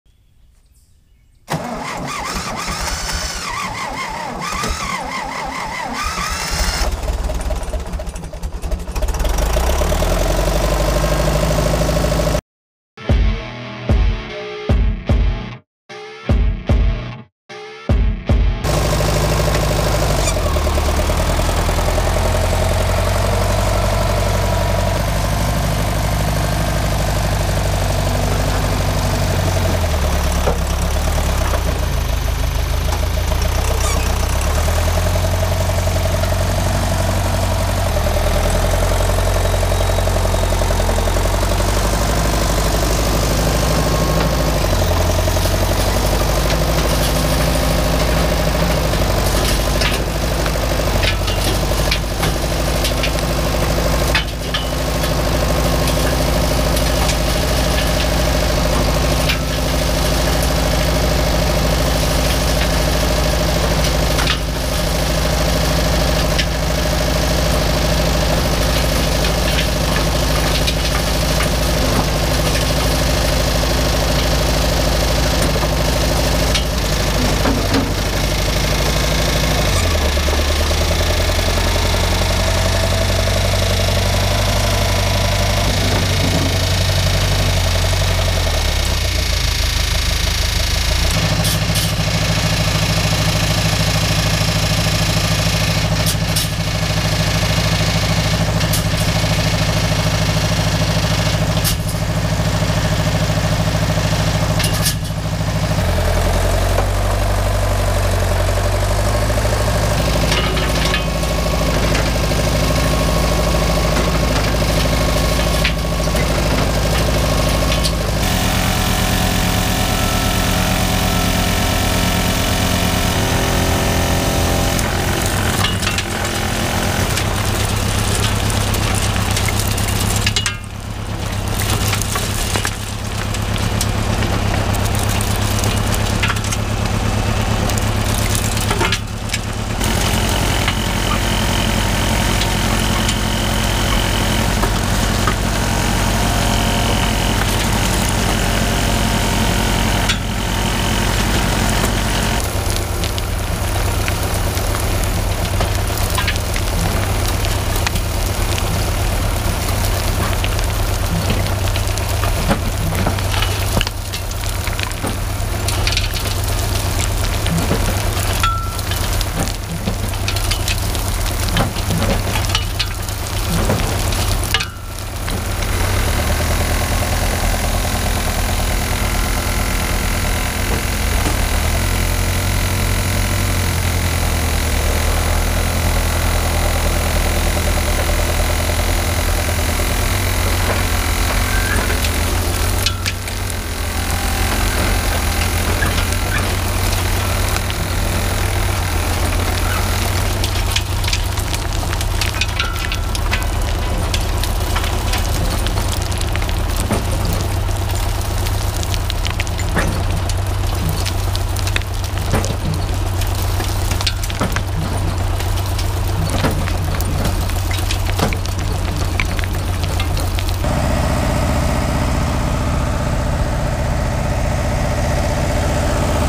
HOMEMADE GRIZZLY SCREENER | SCREENING DIRT
Screening through some dirt with the homemade grizzly screener. Angle iron are welded on for 3 inch minus material. Then the screen on the top is one inch squares for 1 inch minus material using a shaker motor.